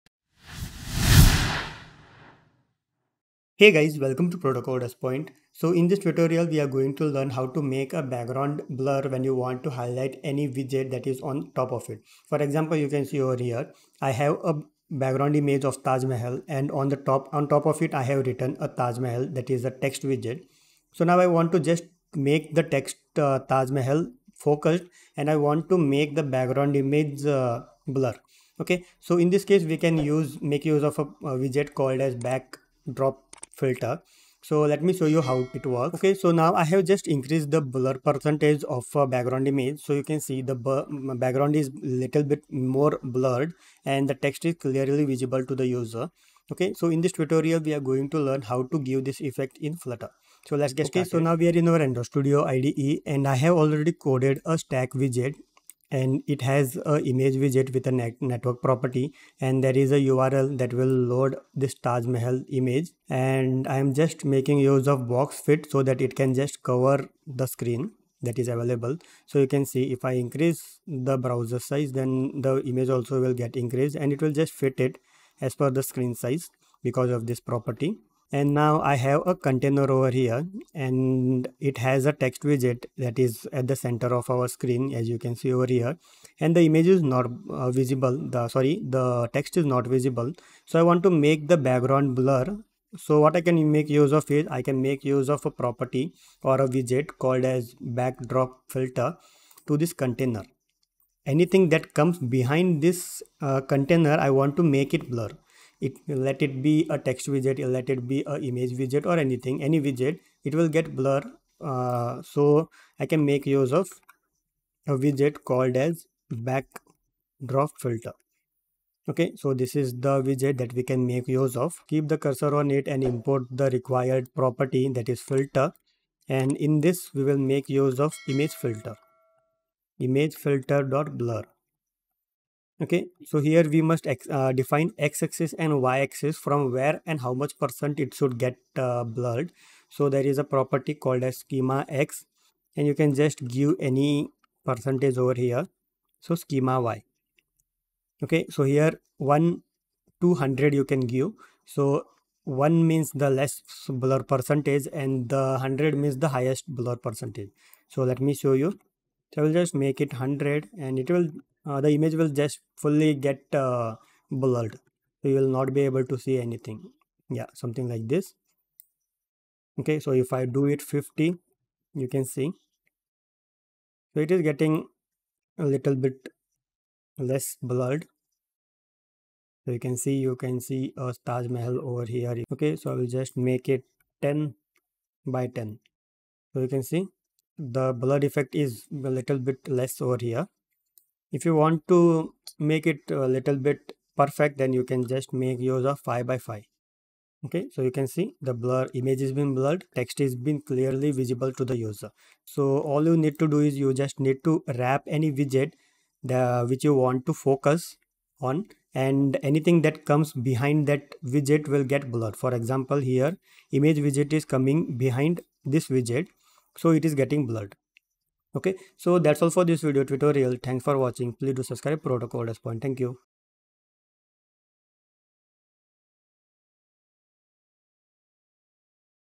Blur Background in flutter using BackdropFilter widget
In this Flutter tutoiral we will learn how to blur the background in Flutter using the powerful BackdropFilter widget. Watch the video now for expert guidance on implementing background blur in your Flutter projects.

Source Code: Soon